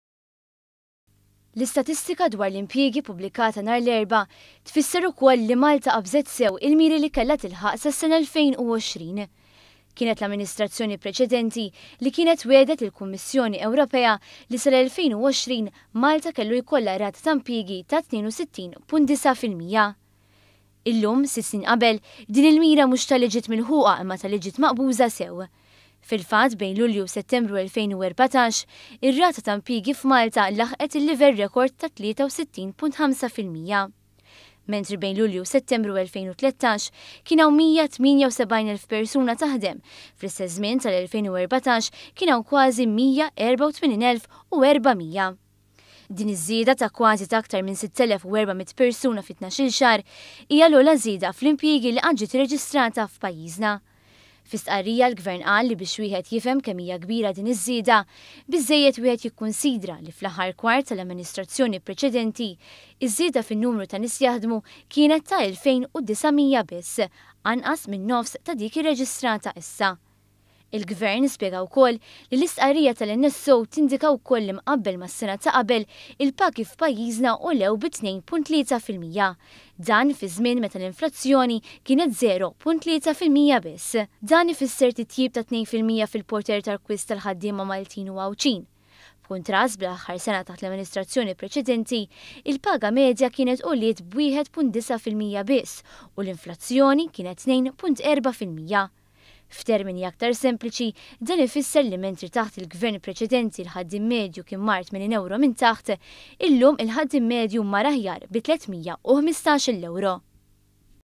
Jinqabżu l-miri li saru mal-Unjoni Ewropea.
L-istatistika ppubblikata llbieraħ dwar il-qasam tax-xogħol tfisser ukoll li miri kellhom jintlaħqu fis-sena elfejn u għoxrin inqabzu sew, sitt snin qabel.